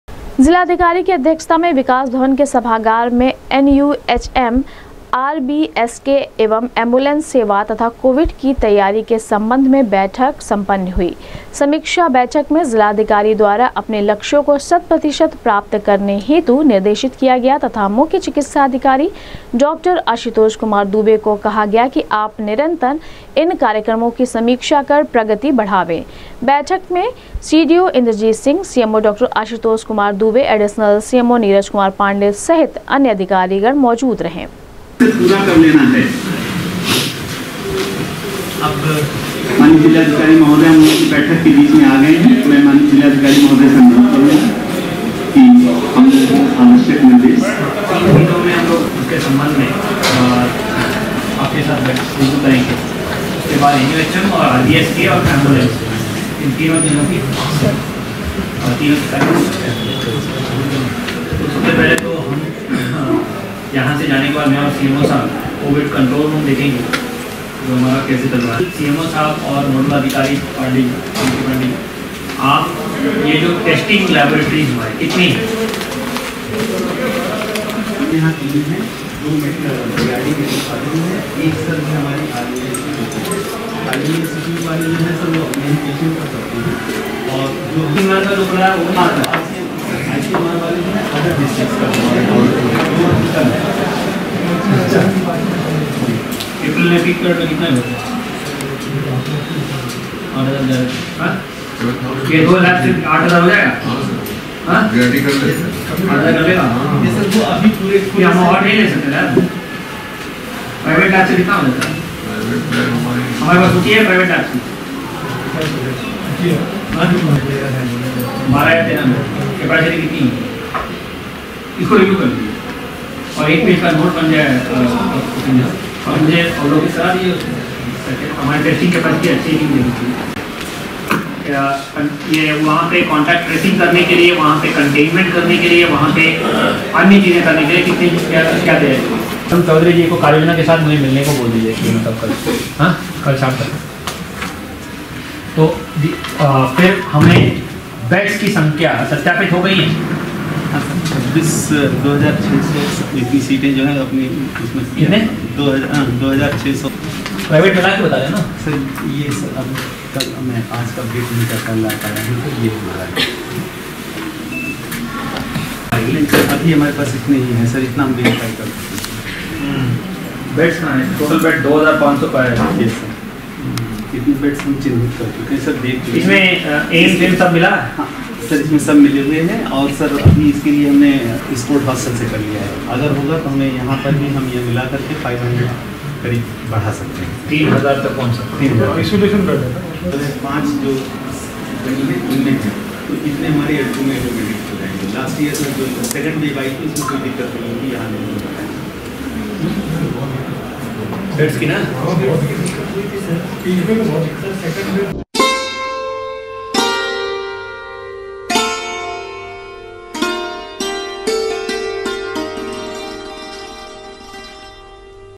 जिलाधिकारी की अध्यक्षता में सम्पन्न हुई एनयूएचएम, आरबीएसके एवं एंबुलेंस सेवा तथा कोविड की तैयारी के
जिलाधिकारी की अध्यक्षता में सम्पन्न हुई एनयूएचएम, आरबीएसके एवं एंबुलेंस सेवा तथा कोविड की तैयारी के संबंध में बैठक, दिये आवश्यक दिशा निर्देश।